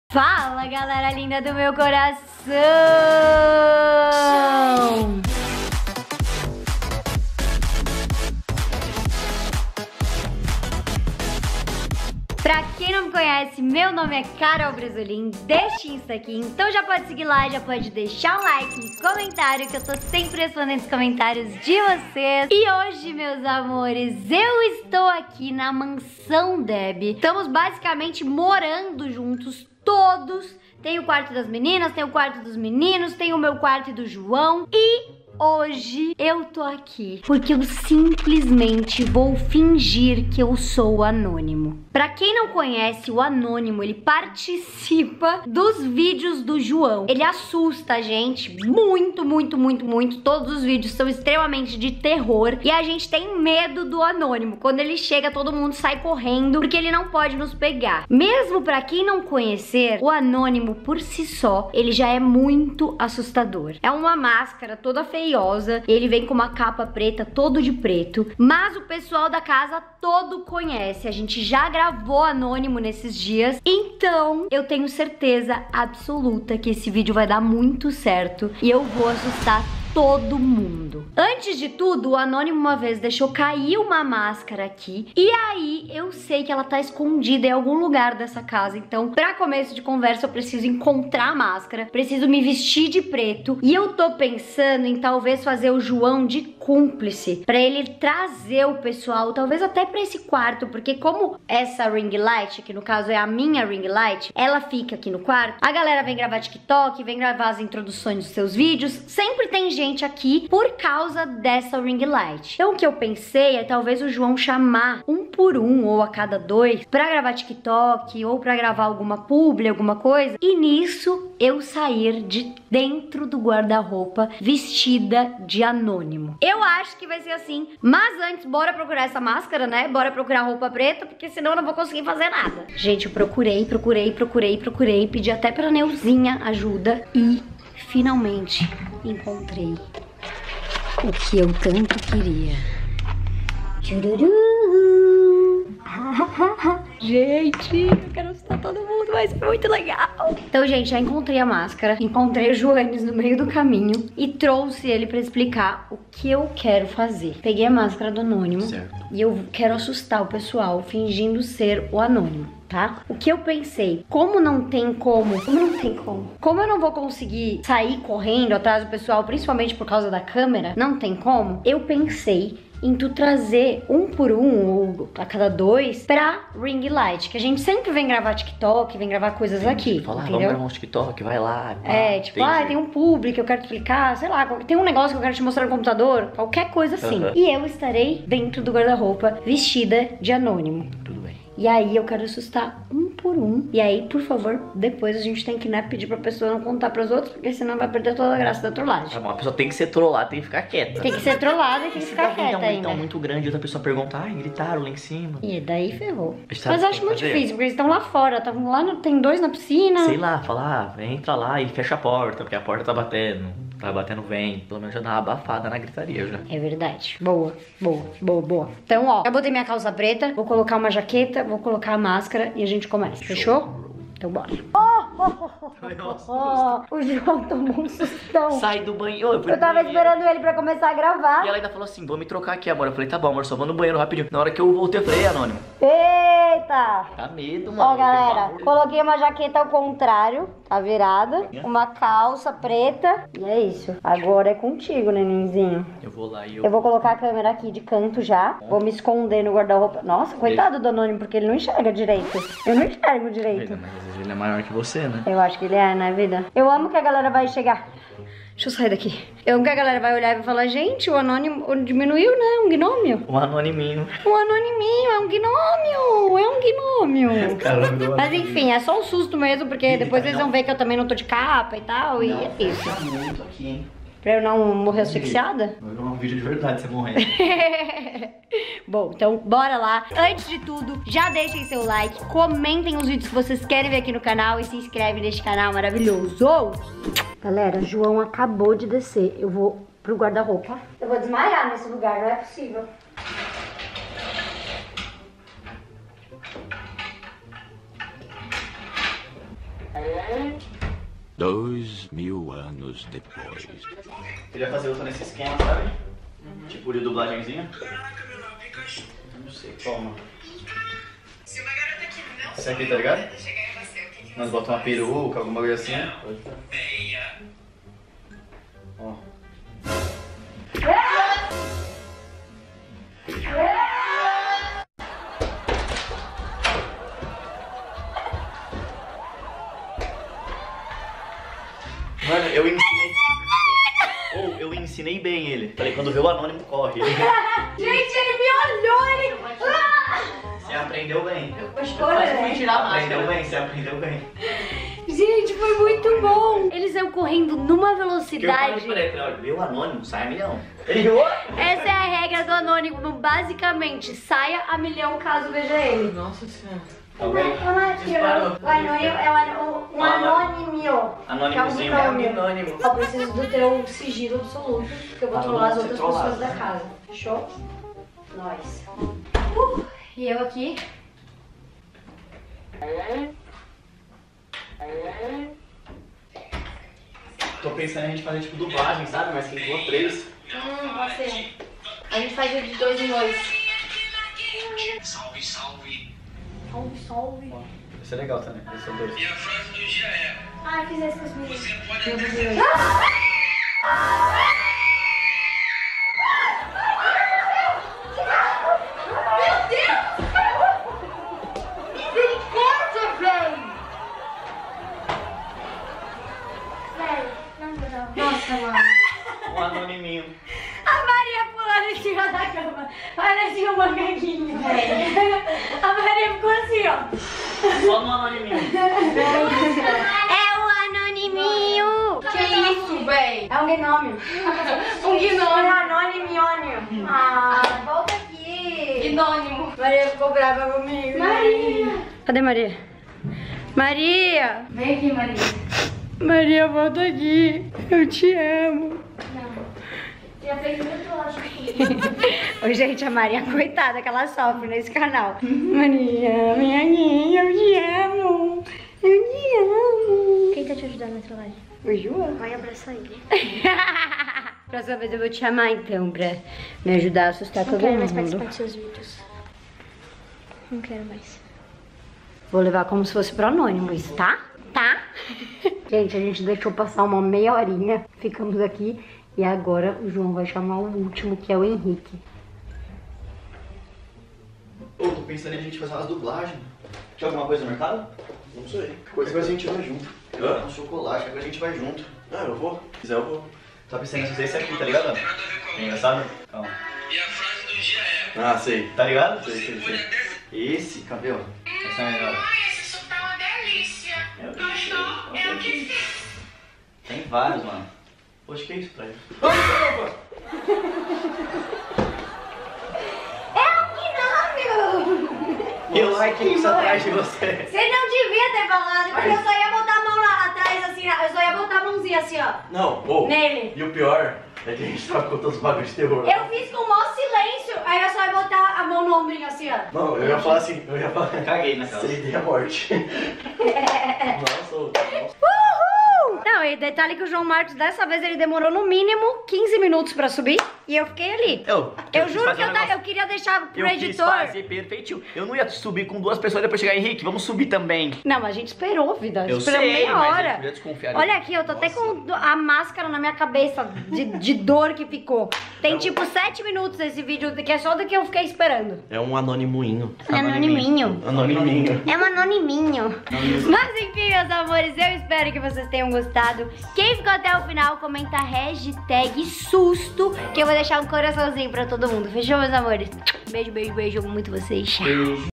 FINGI SER O ANÔNIMO PRA ASSUSTAR ELES E FOI APAVORANTE!!
Coloquei a máscara do anonimo e assustei todos da casa!!
Foi muito apavorante!!